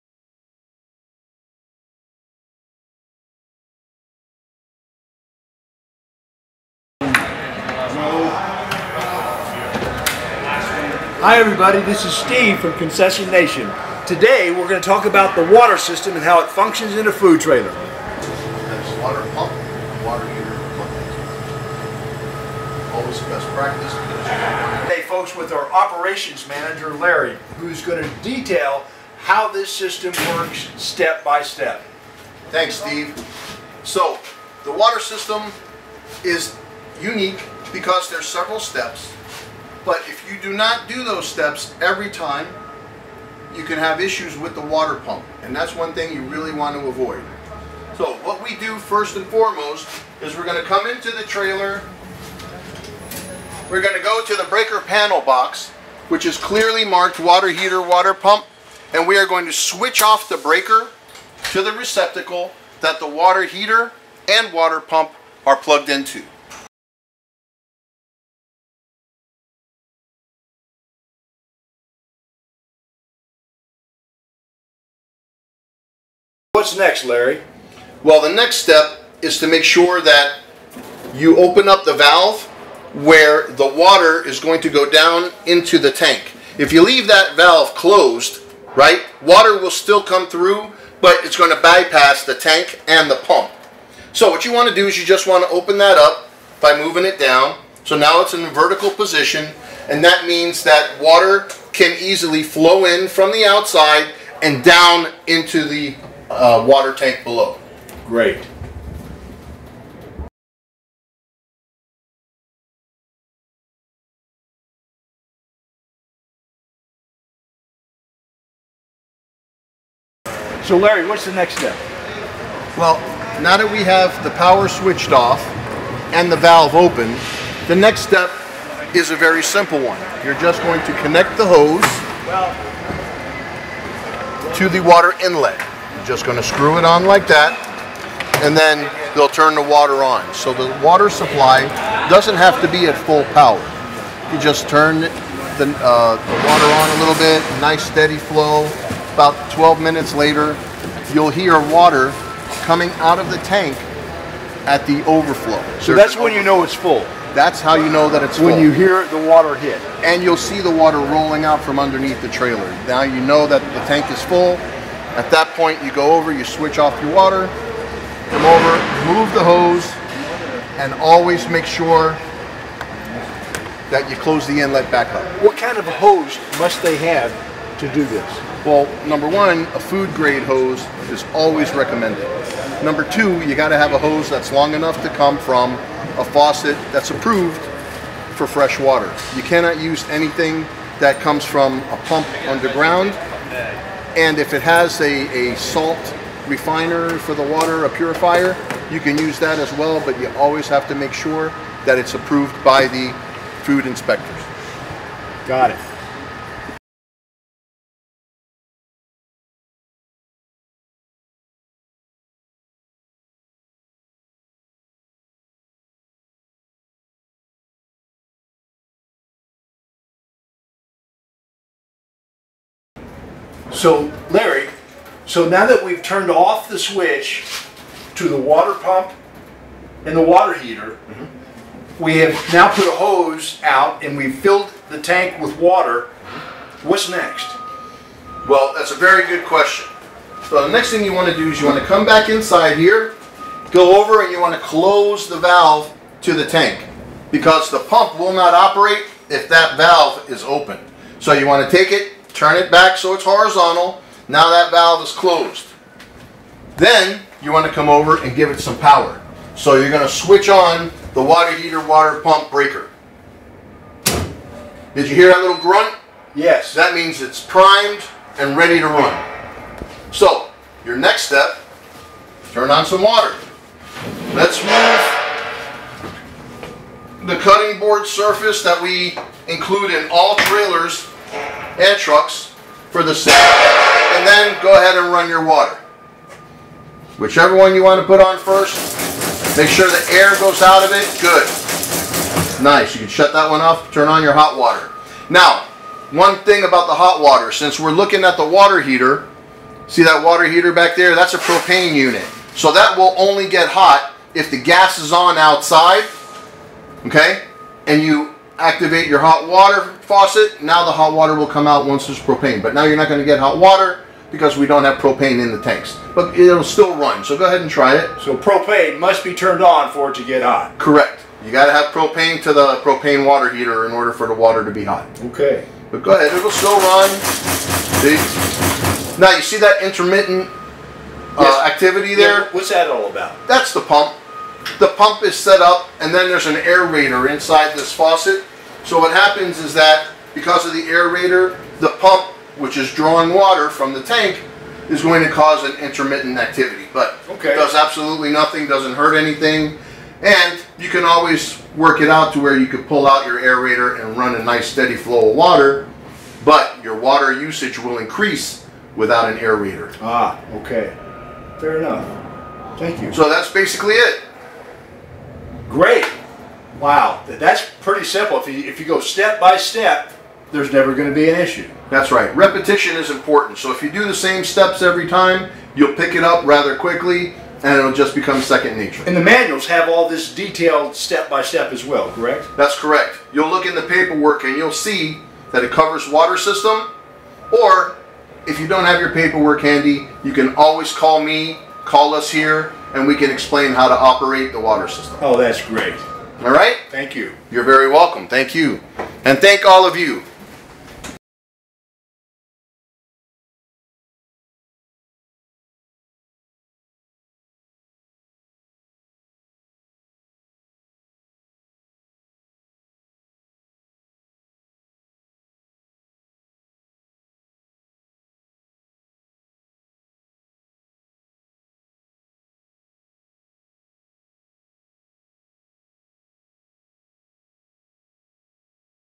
Water Operation System in a Food Trailer
Take a look as Larry and Steve demonstrate on how to operate the water system in a food trailer.